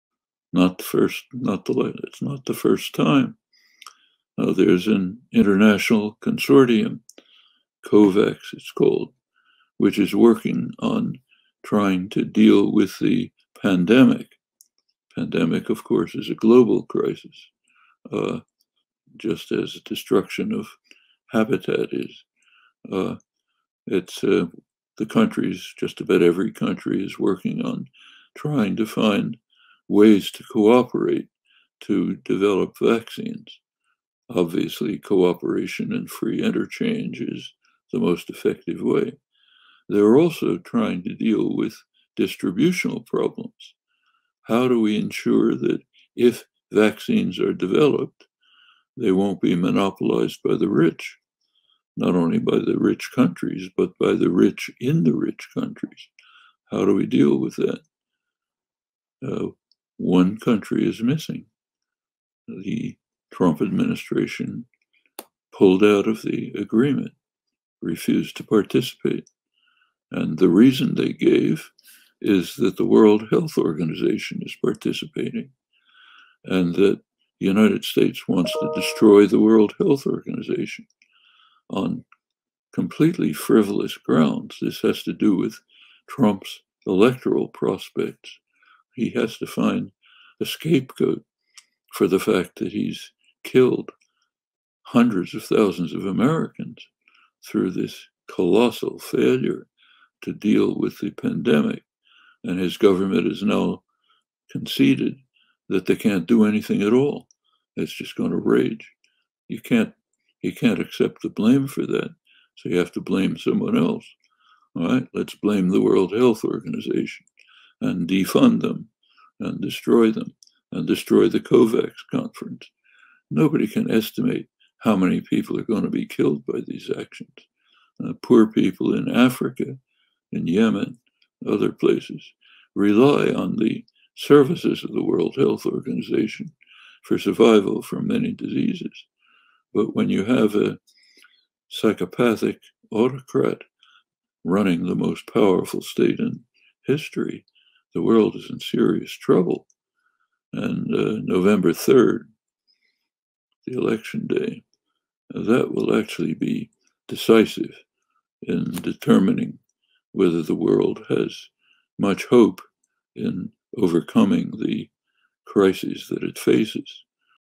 Chomsky Worries about Monopolizing Vaccine Globally l Firoze Alam
Not the first, not the latest not the first time now there’s an international consortium COVAX it's called which is working on trying to deal with the pandemic. Pandemic, of course, is a global crisis just as the destruction of habitat is. It’s the countries just about every country is working on trying to find ways to cooperate to develop vaccines. Obviously, cooperation and free interchange is the most effective way they’re also trying to deal with the distributional problems. How do we ensure that if vaccines are developed they won't be monopolized by the rich, not only by the rich country but by the rich in the rich countries? How do we deal with that? One country is missing.

The Trump administration pulled out of the agreement, refused to participate and the reason they gave is that the World Health Organization is participating. The United States wants to destroy the World Health Organization on completely frivolous grounds. This has to do with Trump’s electoral prospects, he has to find a scapegoat for the fact that he has killed hundreds of thousands of Americans through this colossal failure to deal with the pandemic. His government has now conceded that they can’t do anything at all; it's just going to rage. You can’t accept the blame for that. So, you have to blame someone else. All right, let’s blame the World Health Organization and defund them and destroy them and destroy the Kovac’s conference. Nobody can estimate how many people are gonna be killed by these actions. Poor people in Africa and Yemen and other places rely on the services of the World Health Organization for survival for many diseases but when you have a psychopathic autocrat running the most powerful State in history; the world is in serious trouble.

November the 3rd, the election day will actually be decisive in determining whether the world has much hope in overcoming the crisis that it faces of which we have only mentioned a few, there are others.